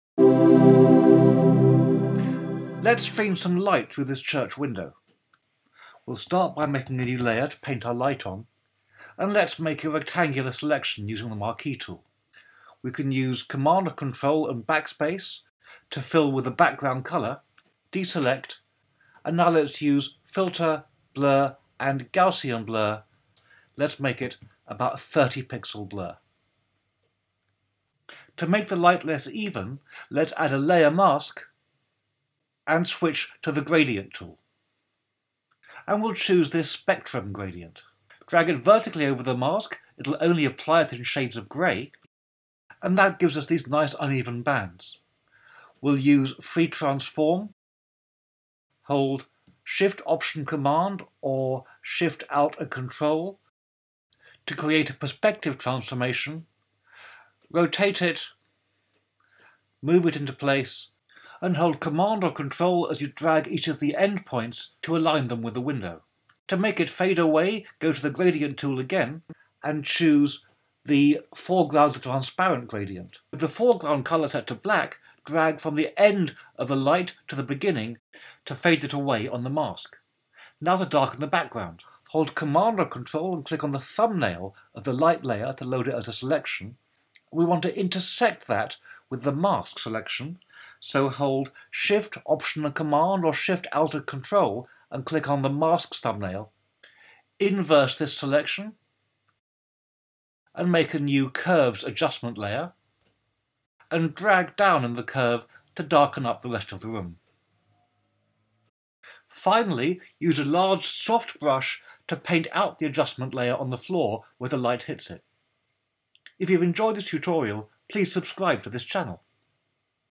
How to stream light through windows
How to use the Gradient Tool and Layer Masks to create the effect of light streaming through a window - in just two minutes. as well as many more free image downloads and tutorials.